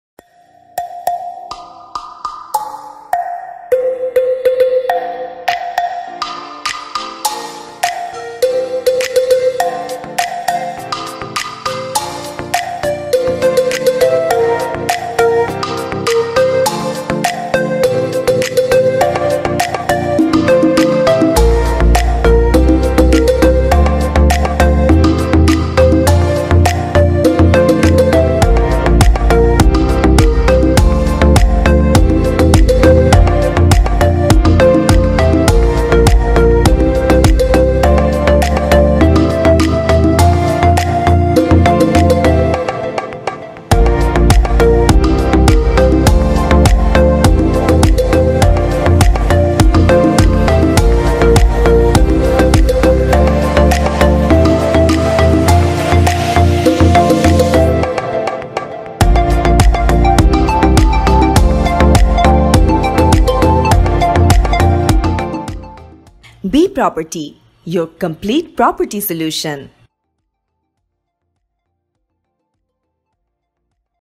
Pleasant 248 Sq. Ft. Office in Motijheel | Commercial Space for Rent in Dhaka
Motijheel is definitely one of those areas when you will find that there are plenty of facilities. The office space here will ensure that there is proper space here and you are able to meet your commercial needs. Since the space is small, it will be easy to maintain and keeping it well arranged will give it a good appearance. for more info or call